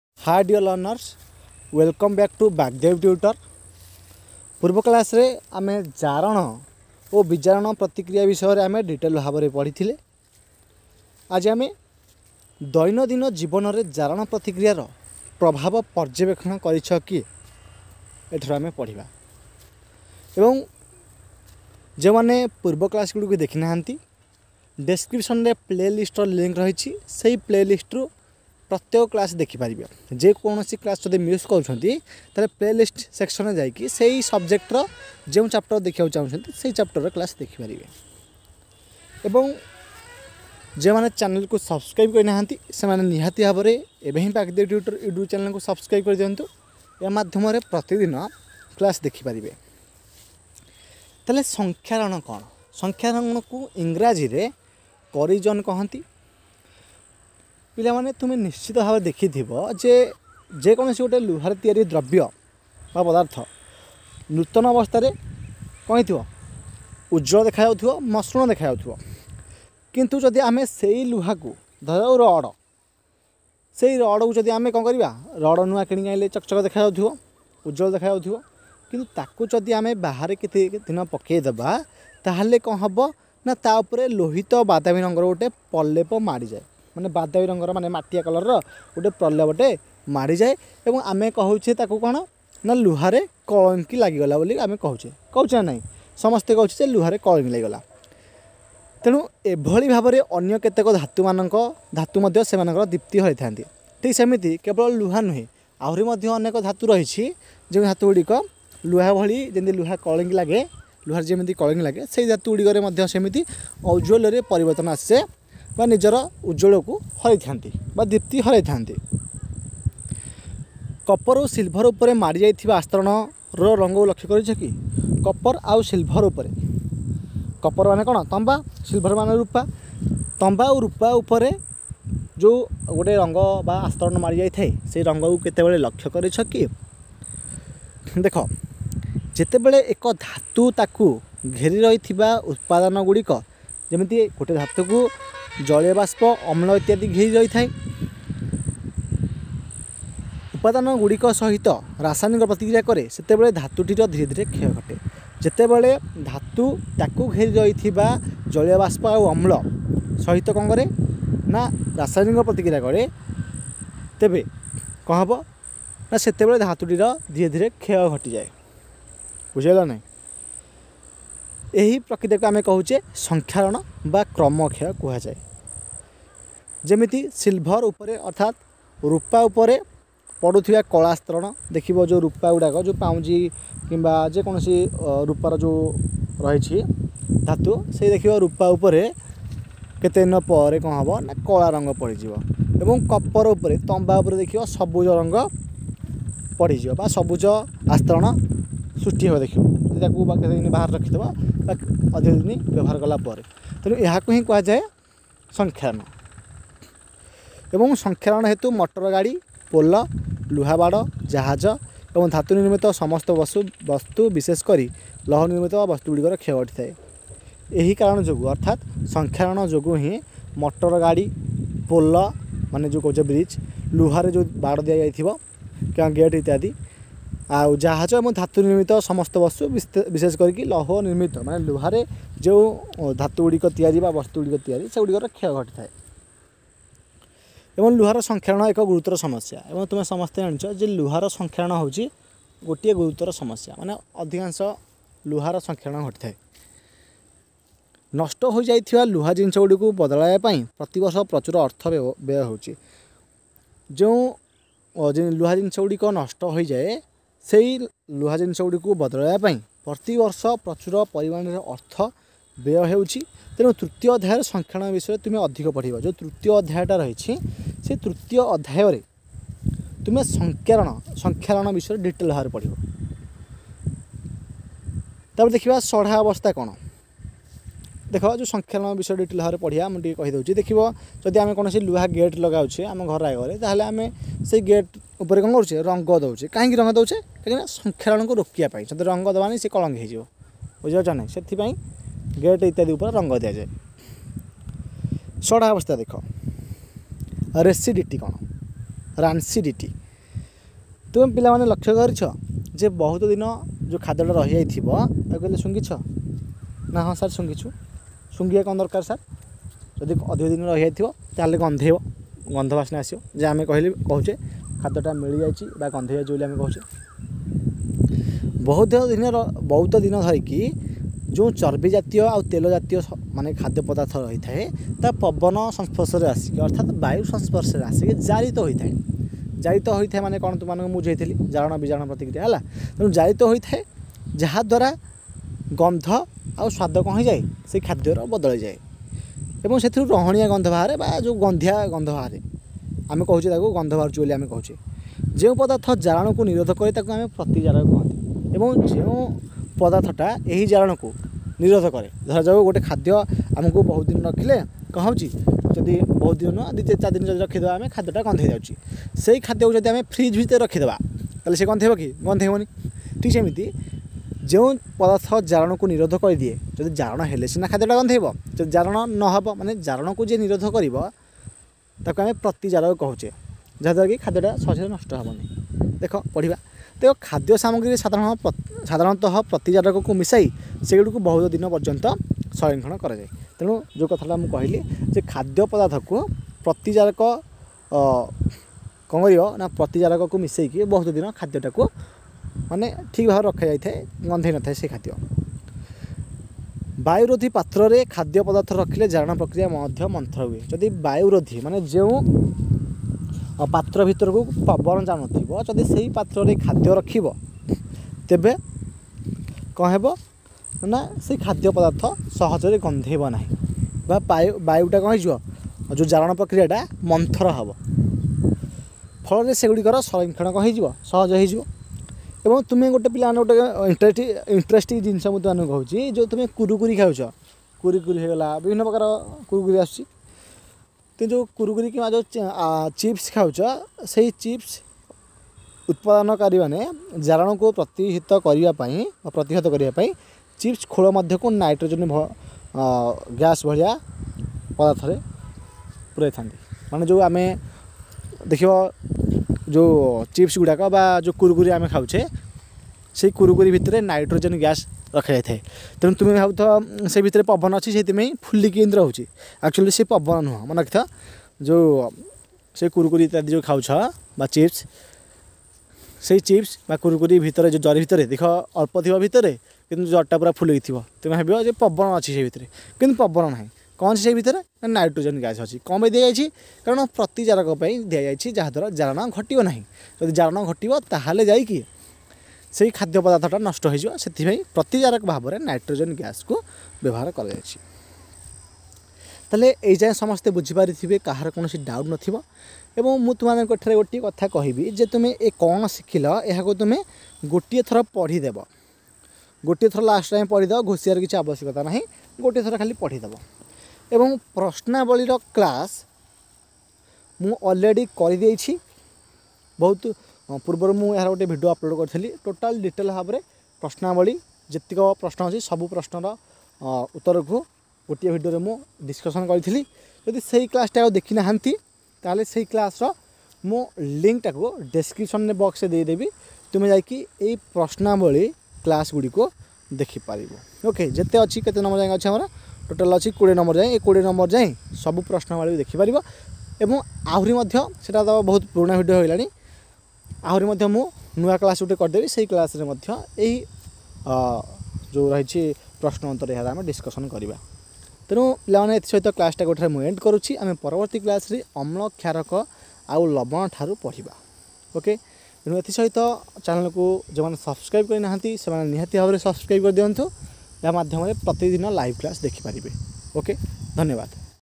10th Class Physical Science Chapter 1 | Chemical Reactions and Equations
ରାସାୟନିକ ପ୍ରତିକ୍ରିୟା ଓ ରାସାୟନିକ ସମୀକରଣ | Chemical Reactions and Equations | Class 10 Science Part-8

10th Class live class odia medium